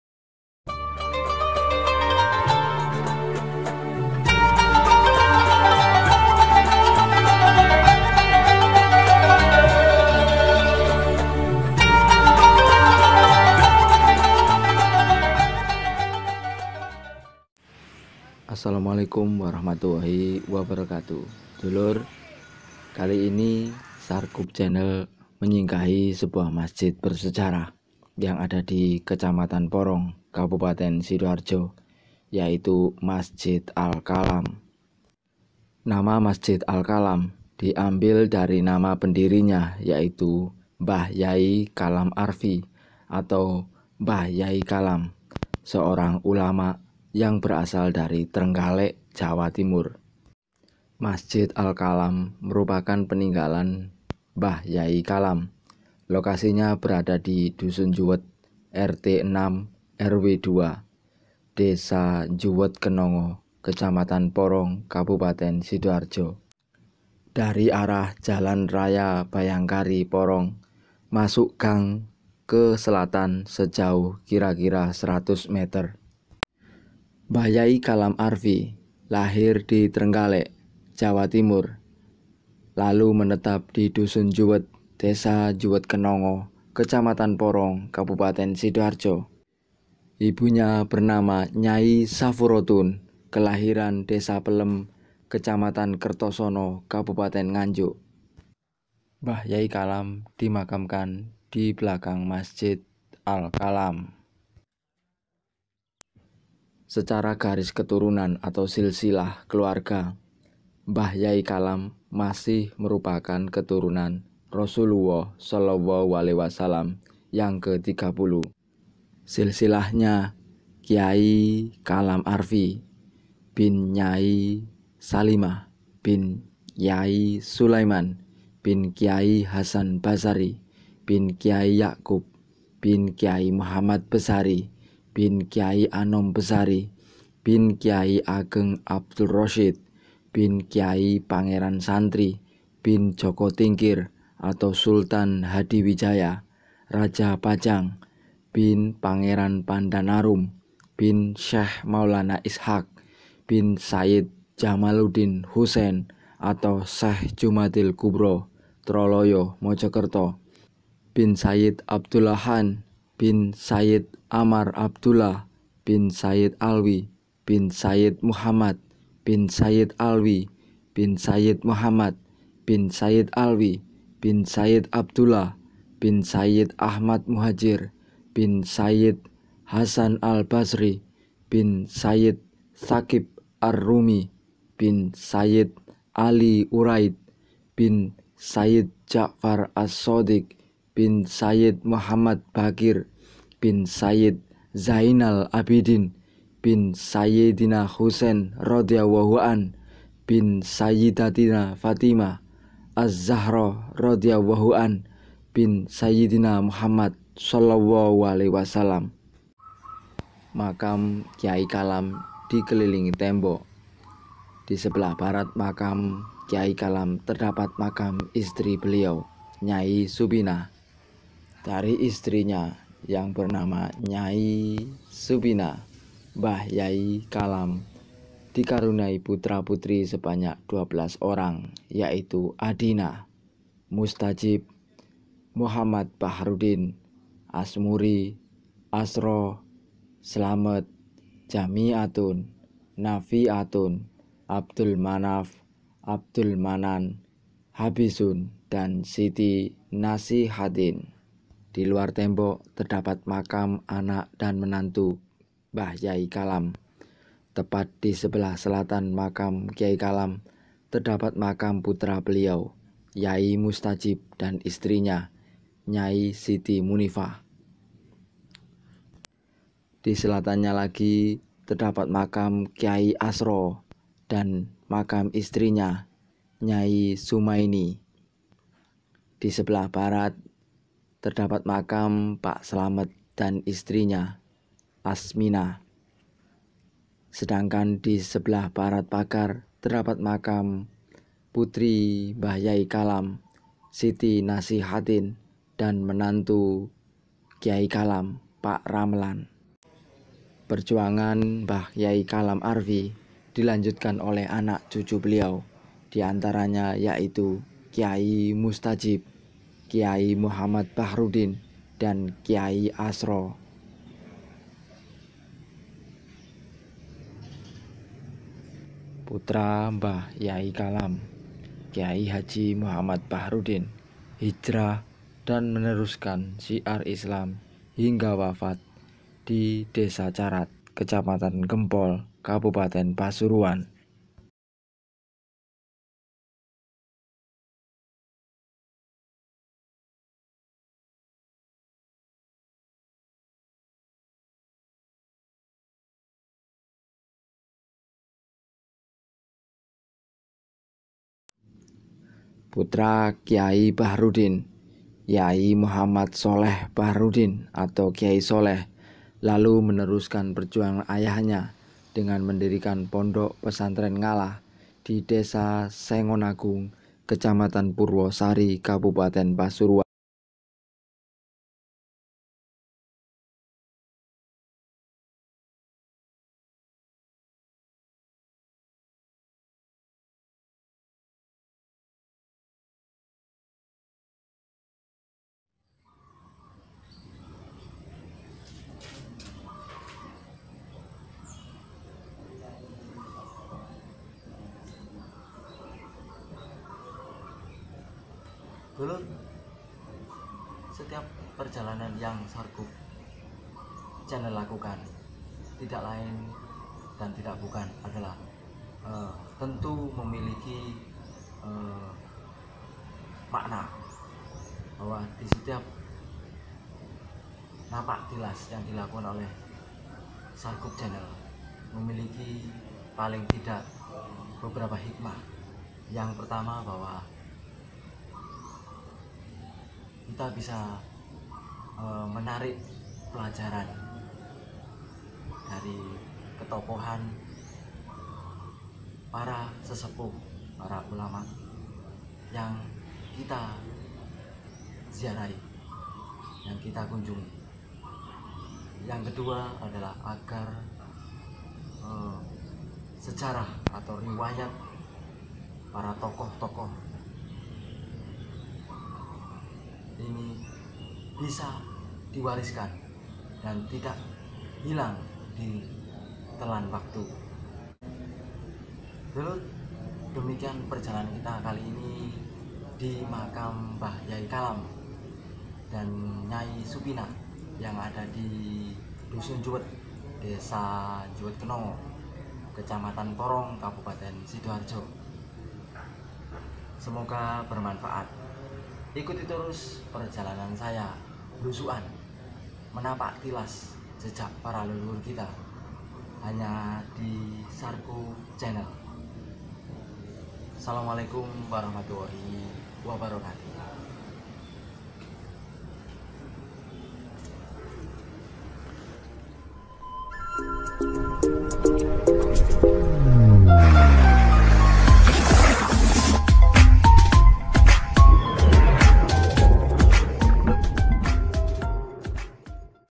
Mbah Kyai Kalam, Kakek Pendiri Ponpes Ngalah
Mbah Kyai Kalam dimakamkan di belakang Masjid Al-Kalam, di Dusun Juwet RT. 6 RW. 02, Desa Juwet Kenongo, Kecamatan Porong, Kabupaten Sidoarjo. Masjid Al-Kalam merupakan peninggalan Mbah Kyai Kalam. Di samping makam Mbah Kyai Kalam terdapat makam istri beliau, Mbah Nyai Supinah. Di luar tembok cungkup terdapat makam anak dan menantu beliau.

Mbah Kyai Kalam Arfi lahir di Trenggalek, lalu menetap di Dusun Juwet, Desa Juwet Kenongo, Kecamatan Porong, Kabupaten Sidoarjo. Ibunya bernama Nyai Safurotun, kelahiran Desa Pelem, Kecamatan Kertosono, Kabupaten Nganjuk.

Dari istri beliau, Nyai Supinah, Mbah Kyai Kalam dikaruniai putra-putri sebanyak 12 orang, yaitu : Atmina, Mustajib, Muhammad Badruddin, Asmuri, Asro, Slamet, Siti Jami'atun, Nafi'atun, Abdul Manaf, Abdul Manan, Habisun, dan Siti Nasichatin.

Perjuangan Kyai Kalam Arfi dilanjutkan oleh anak cucu beliau, di antaranya yaitu Kyai Mustajib, K.H. Muhammad Bahruddin  dan Kyai Asro.

Putra pertama Kyai Bahruddin, K.H. Muhammad Sholeh atau Kyai Sholeh, lalu meneruskan perjuangan ayahnya dengan mendirikan Pondok Pesantren Ngalah di Desa Sengonagung, Kecamatan Purwosari, Kabupaten Pasuruan.

# MOHON MAAF BILA TERDAPAT INFO YANG KURANG / TIDAK SESUAI. VIDEO DIBUAT TIDAK LAIN AGAR KITA TIDAK MELUPAKAN JASA-JASA PARA PENDAHULU KITA. JANGAN SAMPAI KITA KEHILANGAN AKAR SEJARAH.

MBAH KYAI KALAM, THE GRANDFATHER OF THE FOUNDER OF NGALAH ISLAMIC BOARDING SCHOOL

Mbah Kyai Kalam is buried behind the Al-Kalam Mosque, in Juwet Hamlet, RT. 6 RW. 02, Juwet Kenongo Village, Porong District, Sidoarjo Regency, East Java, Indonesia. Al-Kalam Mosque is a relic of Mbah Kyai Kalam. Next to the tomb of Mbah Kyai Kalam there is the grave of his wife, Mbah Nyai Supinah. Outside the wall of the cupola there is the tomb of the child and his son-in-law.

Mbah Kyai Kalam Arfi was born in Trenggalek, then settled in Juwet Hamlet, Juwet Kenongo Village, Porong District, Sidoarjo Regency. His mother is named Nyai Safurotun, born in Pelem Village, Kertosono District, Nganjuk Regency.

From his wife, Nyai Supinah, Mbah Kyai Kalam was blessed with 12 children, namely: Atmina, Mustajib, Muhammad Badruddin, Asmuri, Asro, Slamet, Siti Jami'atun, Nafi'atun, Abdul Manaf, Abdul Manan, Habisun and Siti Nasichatin.

The struggle of Kyai Kalam Arfi was continued by his grandchildren, including Kyai Mustajib, K.H. Muhammad Bahruddin and Kyai Asro.

The first son of Kyai Bahruddin, K.H. Muhammad Sholeh or Kyai Sholeh, then continued his father's struggle by establishing the Islamic Boarding School Islamic Boarding School in Sengonagung Village, Purwosari District, Pasuruan Regency.